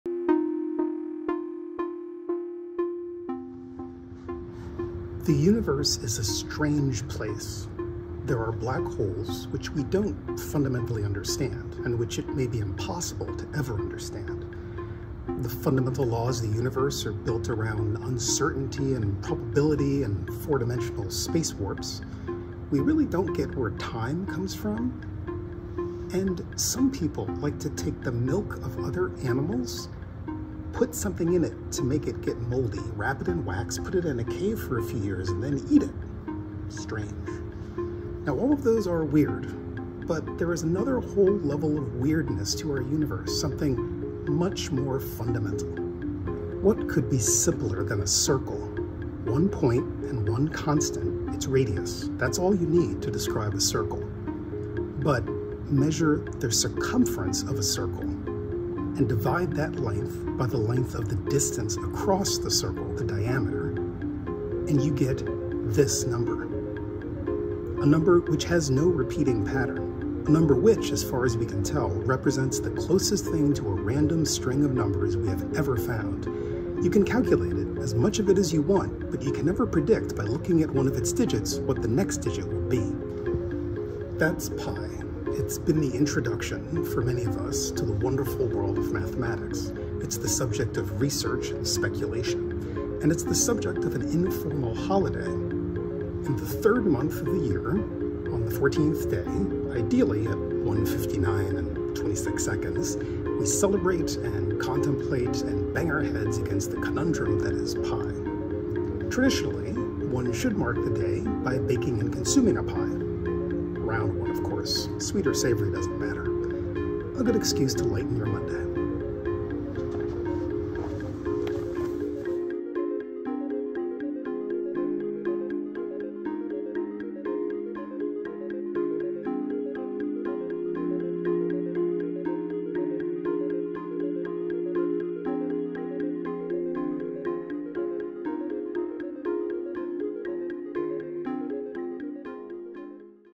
Happy Pi Day 2022!
Happy PiDay!! But, wait… what is Pi, exactly, other than a tasty dessert? What does it mean, and where does it come from? Join Marc Taylor, Manager of Planetarium and Science Programs at the HRM, as he explains the inherent oddness of Pi - or the ratio of a circle's circumference to its diameter - in this video. We hope you have a lovely Pi day, and that you get the chance to bake a pie in celebration of this strange mathematical phenomenon!

Want to learn more about current astronomical and cosmic events? Come to “The Sky Tonight,” every Saturday and Sunday at 2pm!